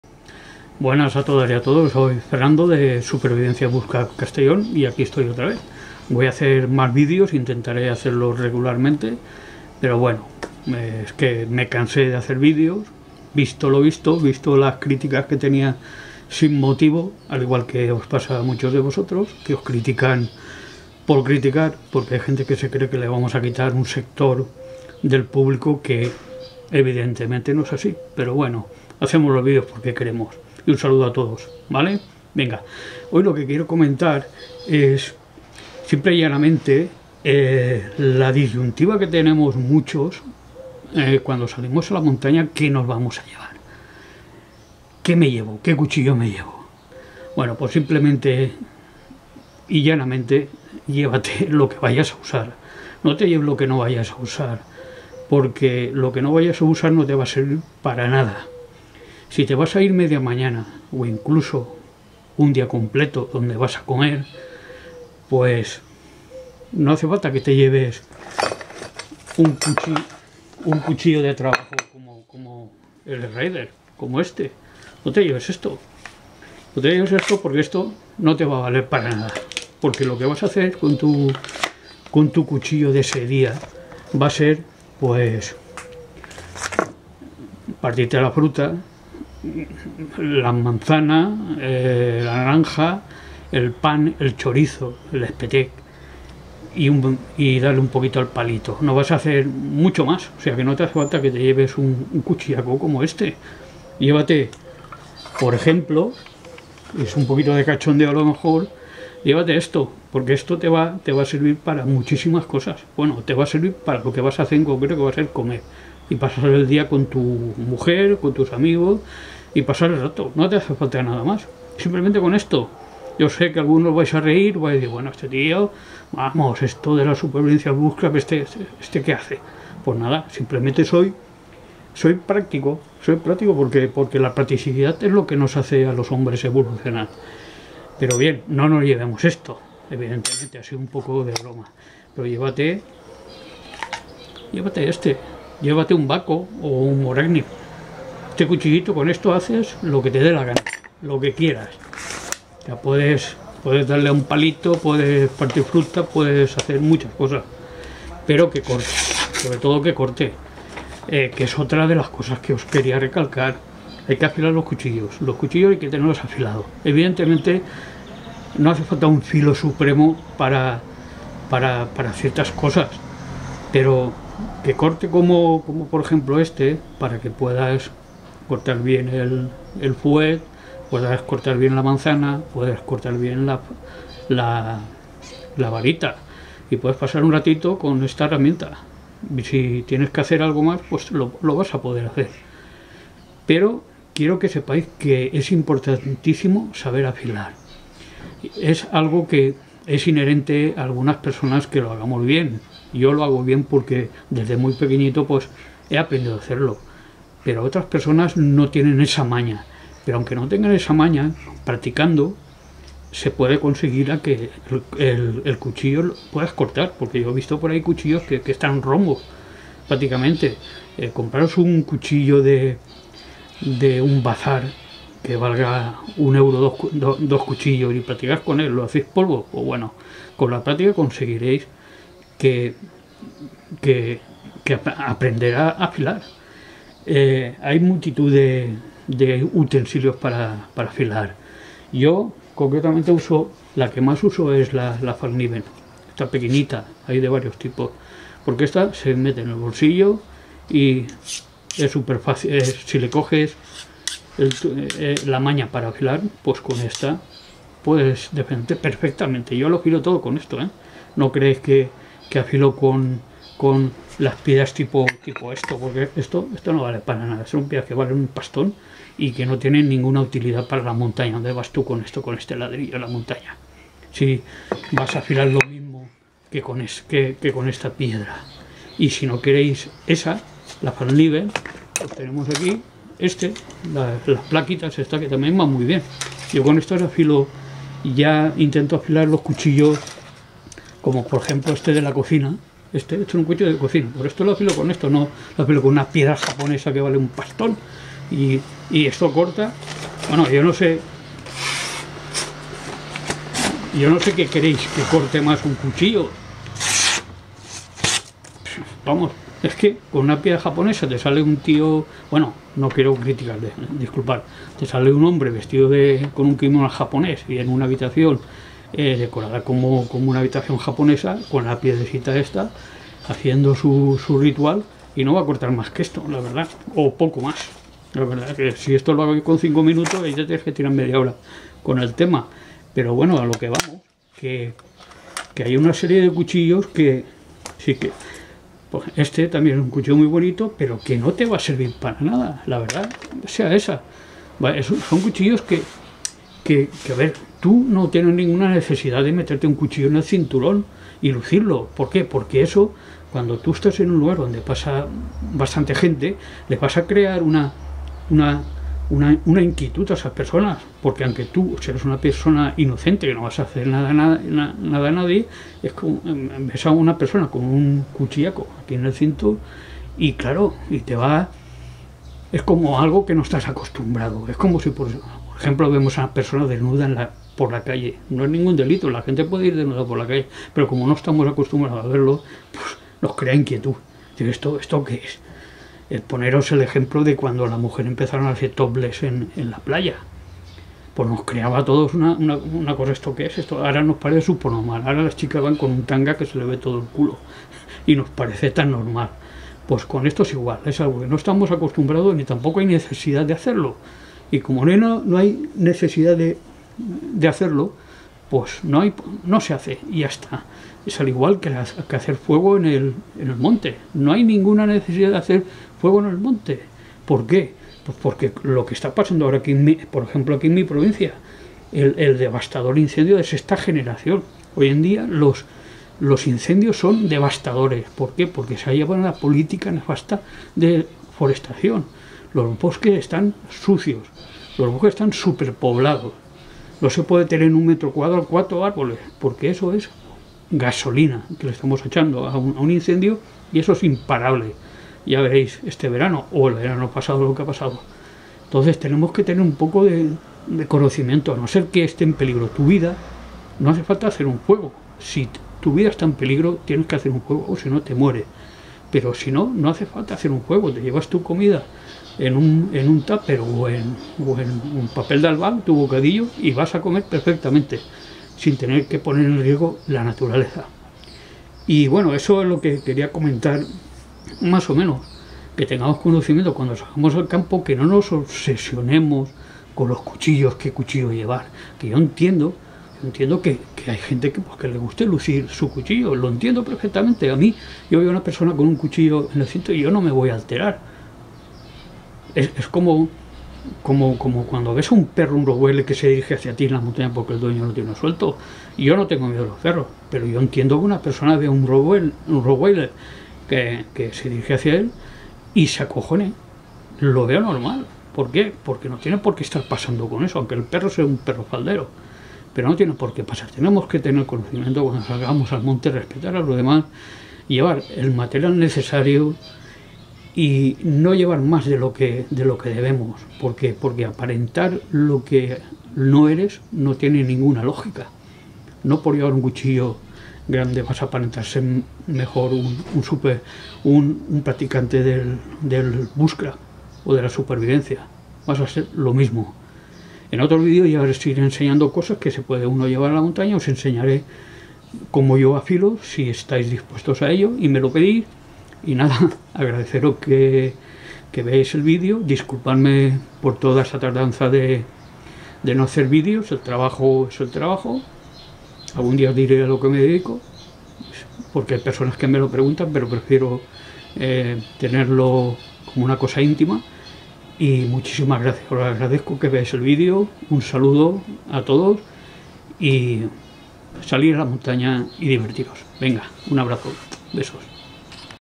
El CUCHILLO PERFECTO para tu aventura en la MONTAÑA: excursiones de un dia.el MEJOR CUCHILLO.
¿Estás planeando una excursión de un día a la montaña y no sabes qué cuchillo llevar?

En este video, te guiaré a través del proceso de elegir el cuchillo perfecto para tu aventura, tomando en cuenta factores como el peso, el tamaño, la versatilidad y la seguridad.
Te presentaré una  selección de los mejores cuchillos para salidas de un día a la montaña, comparando sus características y ventajas.
Además, te enseñaremé:
Cómo elegir el tamaño y el peso adecuados para tu mochila.
Qué tipo de hoja es la mejor para tus necesidades.
Qué características adicionales debes considerar.
Cómo cuidar y mantener tu cuchillo en la montaña.
Con esta guía, estarás seguro de elegir el cuchillo perfecto para tu próxima aventura en la montaña.
Este video es ideal para:
Excursionistas de un día
Amantes de la naturaleza
Aventureros
Cualquiera que quiera disfrutar de un día seguro y agradable en la montaña

Recuerda que puedes adaptar esta descripción a tu estilo y a la audiencia que deseas llegar.